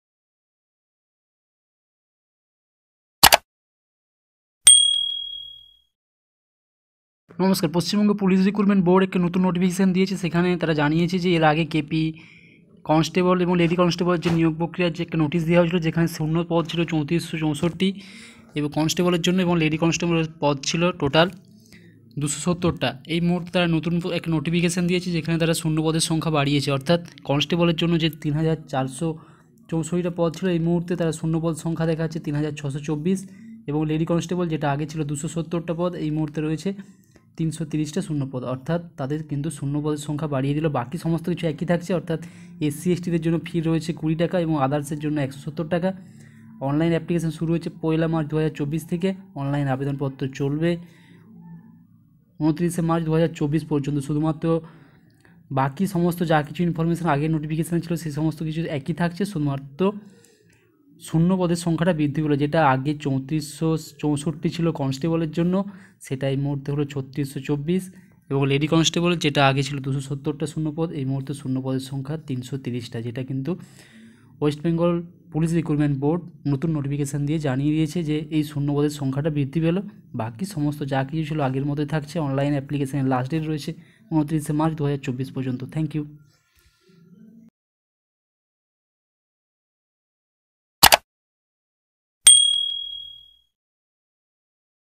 Kolkata Police Recruitment 2024 । শূণ্যপদ বৃদ্ধি পেল । KP Constable recruitment
শূণ্যপদ বৃদ্ধি পেল 👇👇👇
কনস্টেবল 3624
লেডি কনস্টেবল 330

বাকি সমস্ত তথ্য একই রয়েছে।
আবেদন ফি 👇👇
SC/ST Rs 20/-
Others Rs 170/-

Online Application Last date: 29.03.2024